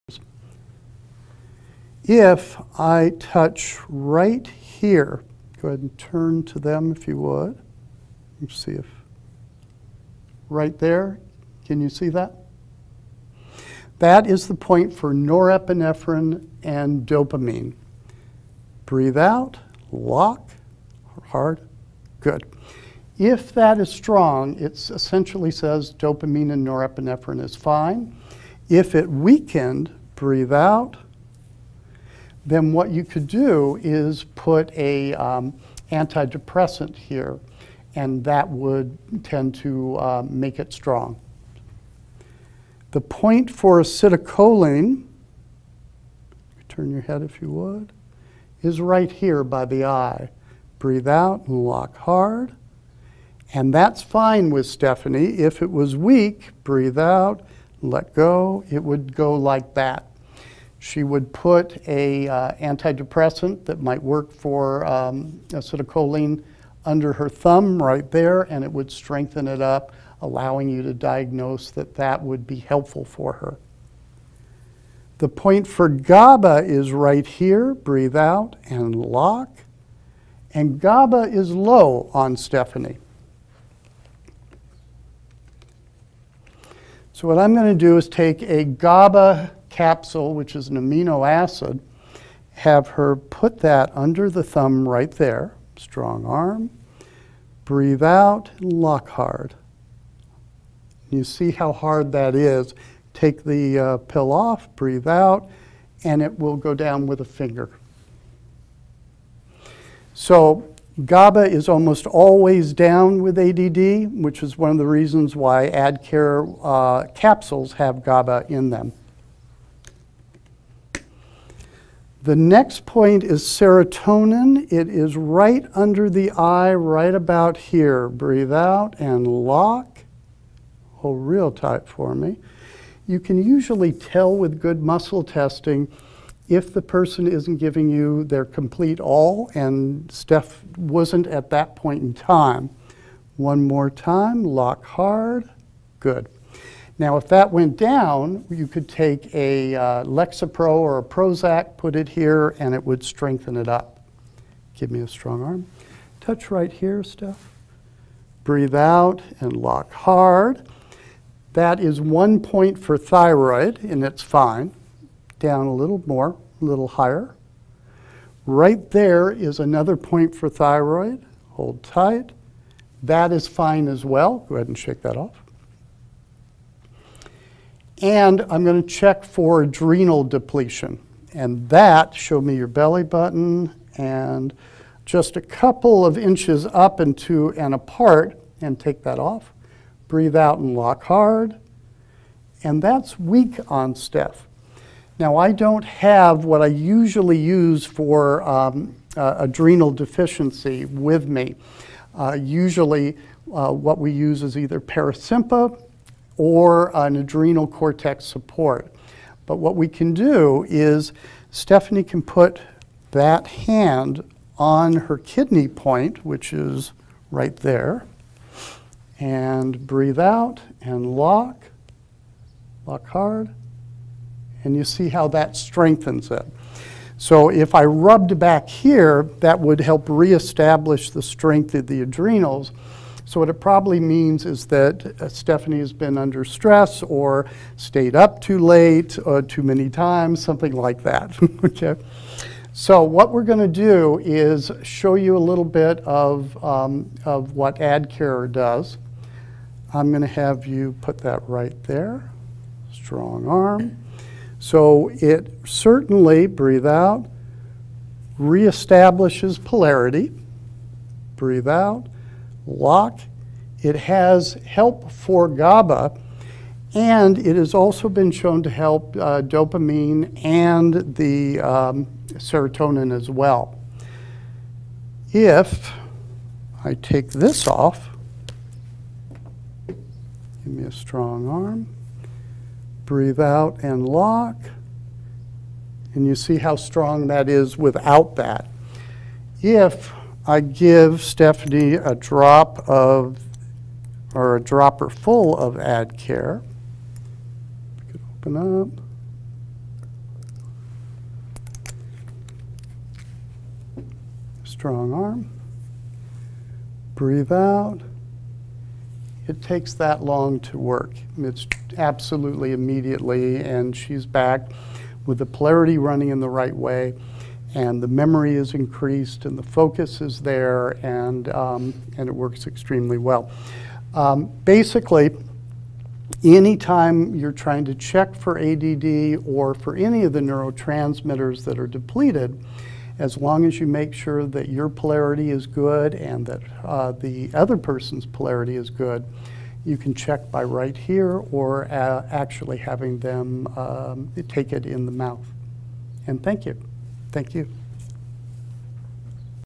AK or Applied Kinesiology - Muscle Testing - Part 2
Jef Gazley, M.S., LMFT demonstrates how to perform AK or Applied Kinesiology. This is the preamble to learning how to diagnose ADD (Attention Deficit Disorder) or ADHD (Attention Hyperactivity Deficit Disorder) using Applied Kinesiology.  AK can also be used to determine what kind of medication, whether natural or prescription, can be use to treat ADD or ADHD.